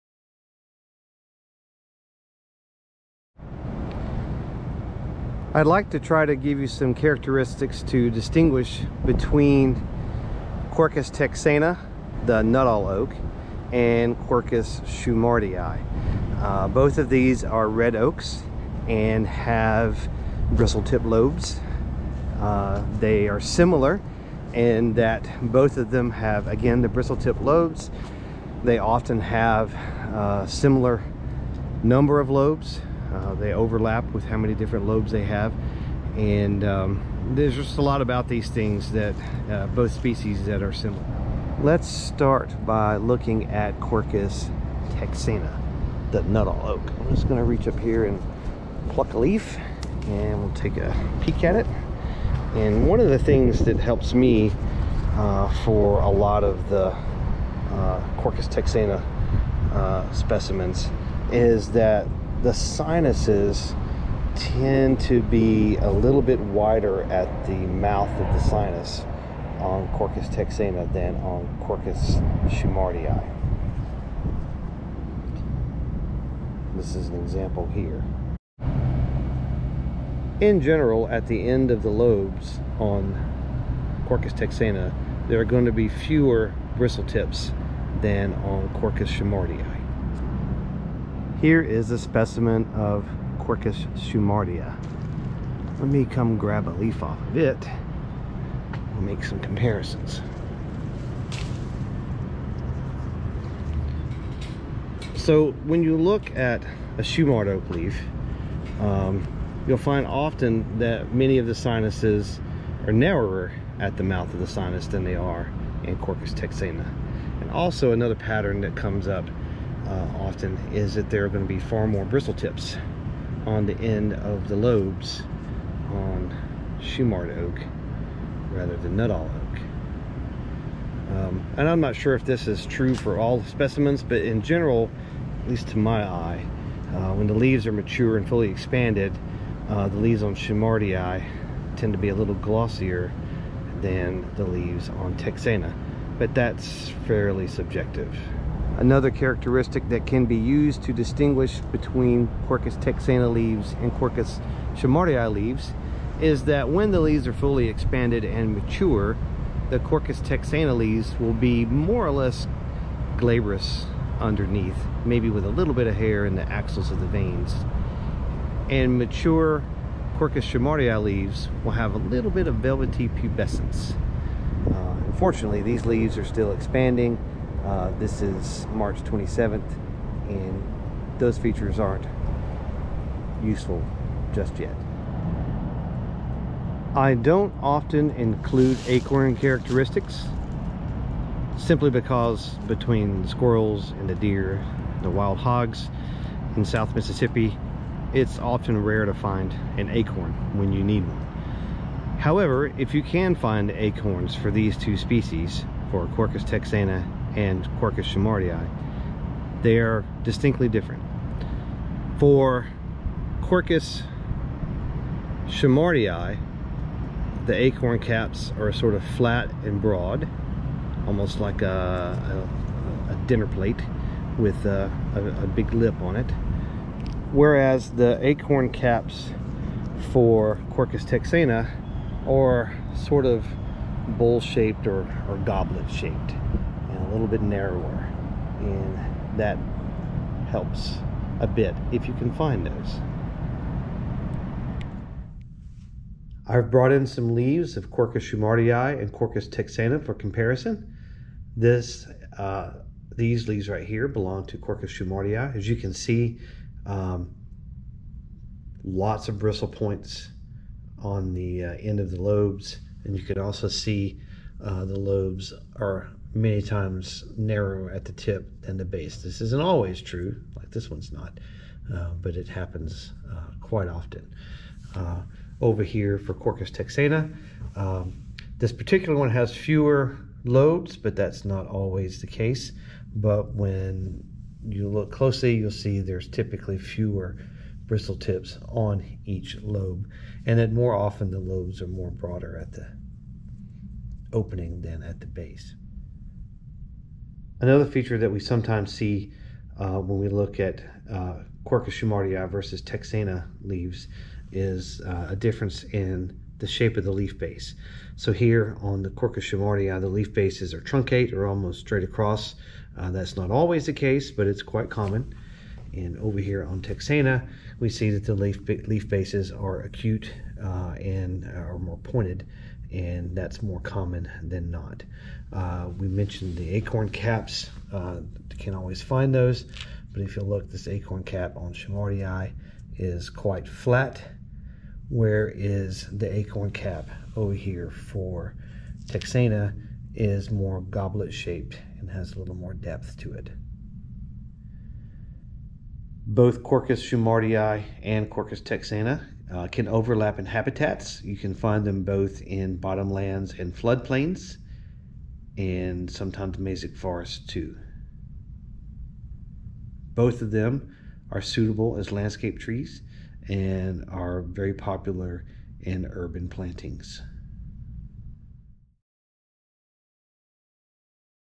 Quercus shumardii vs Quercus texana
Quercus shumardii and Quercus texana are both red oaks with bristle tipped lobes that can overlap in habitat.  Here we try to use leaves, bark, and acorns to distinguish these species from each other.